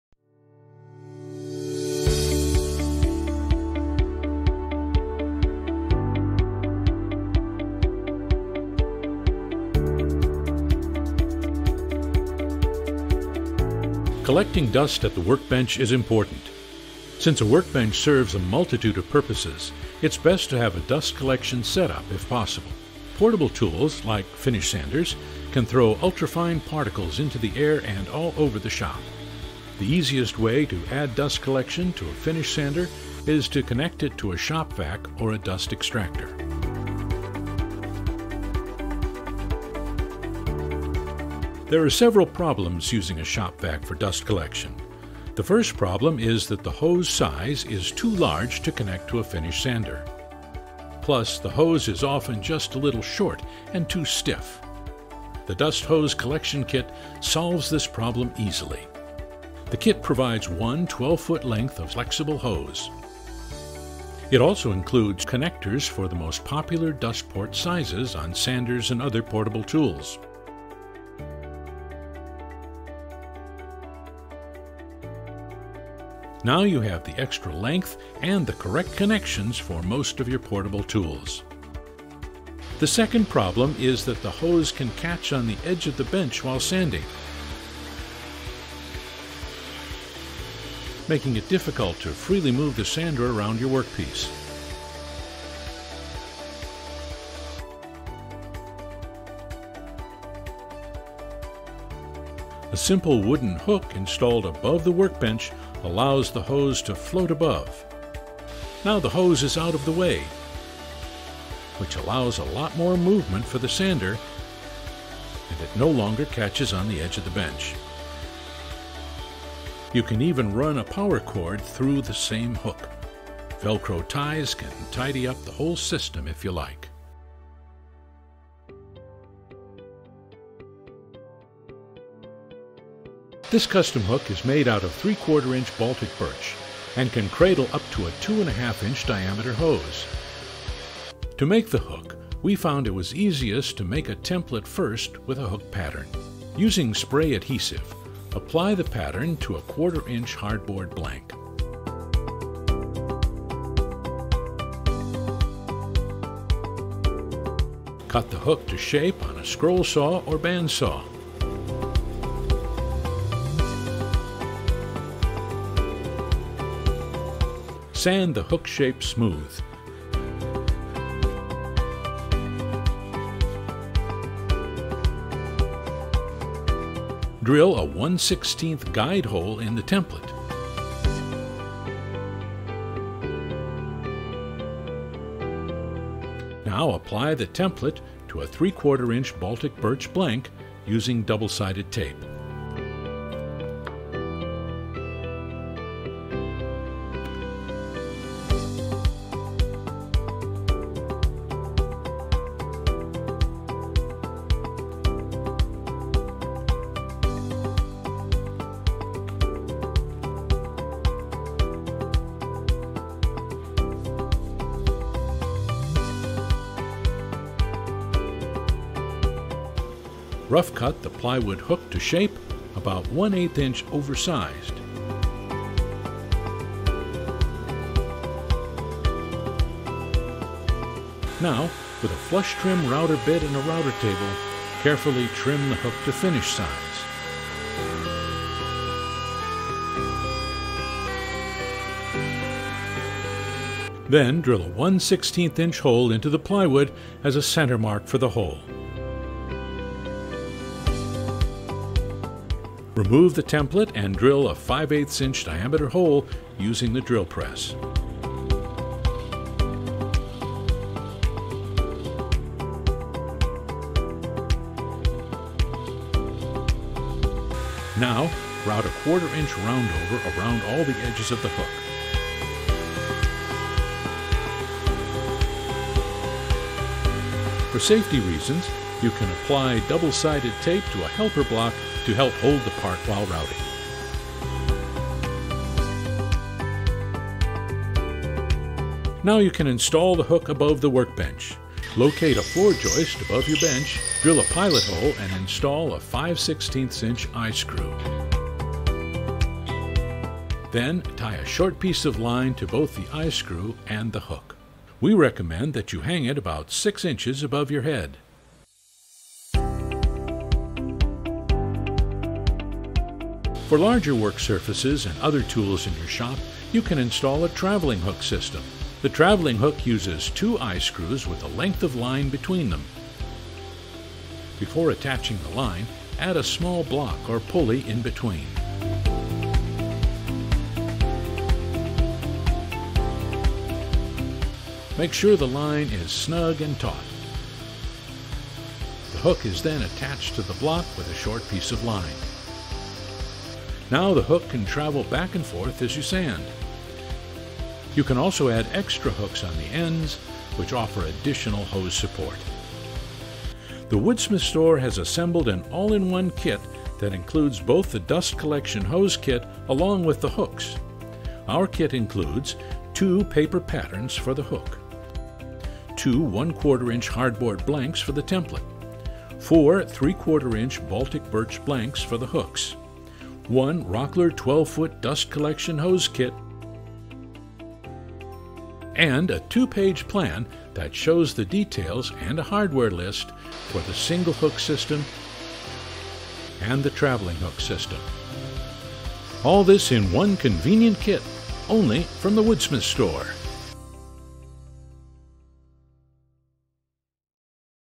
Dust Collection Solutions at the Bench
Setting up dust collection at the workbench can be a challenge. Portable tools like finish sanders have tiny ports, but shop vacs have large hoses. Plus a dust collection hose can drag across the edge of the bench and the workpiece, making it difficult to use your tools. Let’s take a look at a hose kit and a ceiling hook that make it work much better!

Get more tips like these + $25 in FREE online coupons, good for online purchases.

The Woodsmith Store Fine Tools Dust Collection Hose Hook